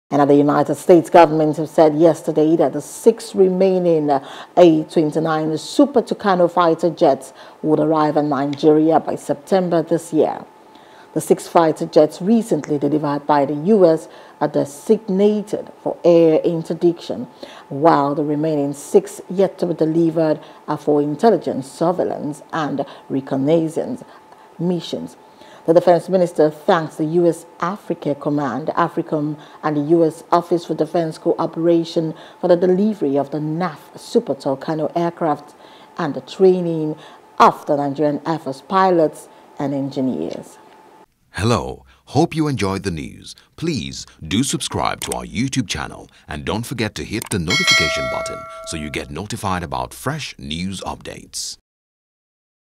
Anti-Insurgency: Nigeria To Receive Six Super Tucano Fighter Jets In September | NEWS
The United States Government said yesterday that the six remaining A-29 Super Tucano fighter jets would arrive in Nigeria by September this year.